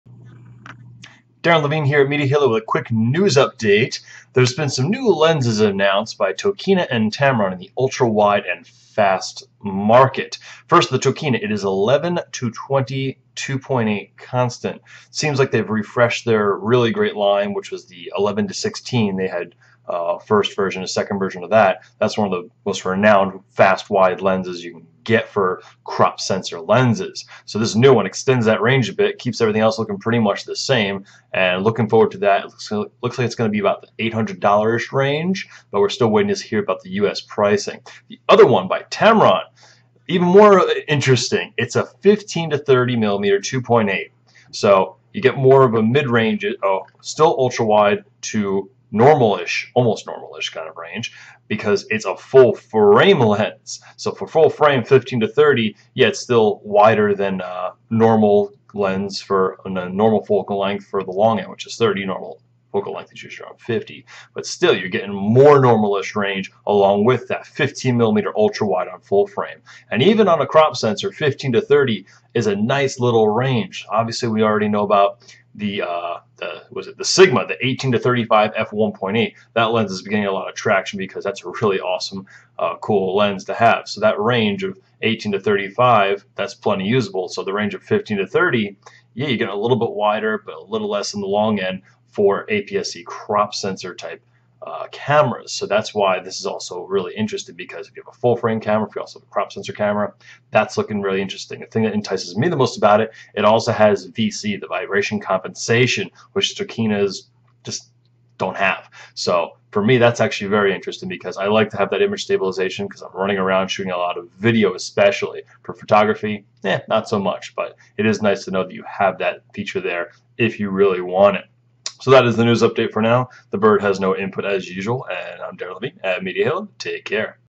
News: Two ultra-wide & Fast lenses to watch out for: 11-20mm & 15-30mm, Both F2.8
The fast ultra wide market is not quite as populated as the rest of the lens market, so when something interesting comes out, it gets me plenty excited. The Sigma 18-35mm 1.8 became the fastest crop sensor zoom, and stores have trouble keeping them in stock. Now we have two new interesting pieces of glass to look at.